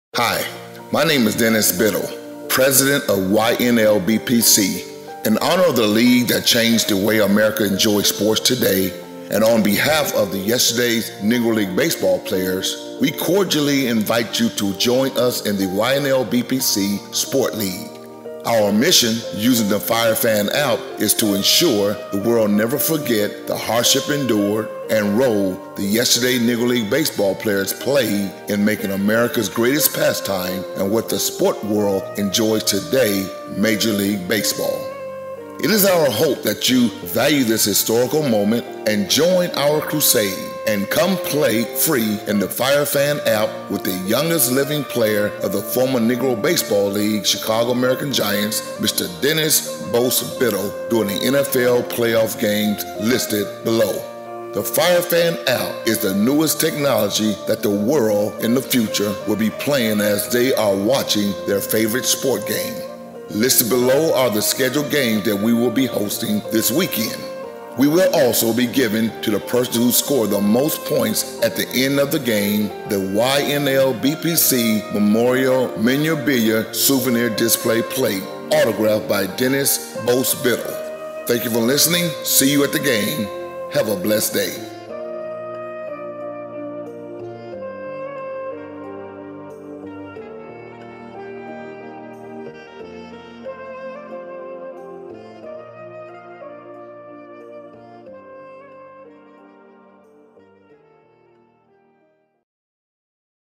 Play Firefan in the YNLBPC Players League with Dennis "Bose" Biddle
In honor of the league that changed the way America enjoy sports today, the "Negro Baseball League"; click link to join the Youngest living player of the former Negro league, Chicago American Giants Dennis "Bose" Biddle in the YNLBPC Players Challenge FireFan Game.

Thank you, see you in the FireFan game

For more information and to join the YNLBPC Players League